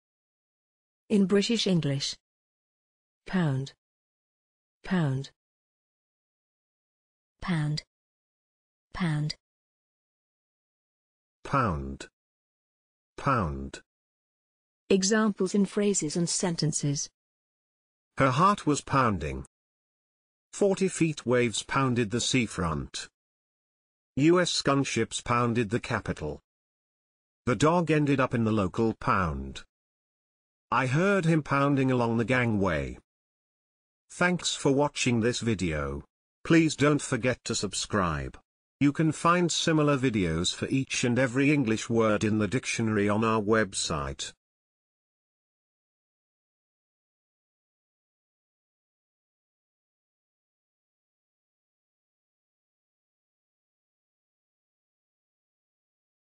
pound - pronunciation + Examples in sentences and phrases
This is the pronunciation of pound in British English + a few examples of its pronunciation in phrases and sentences.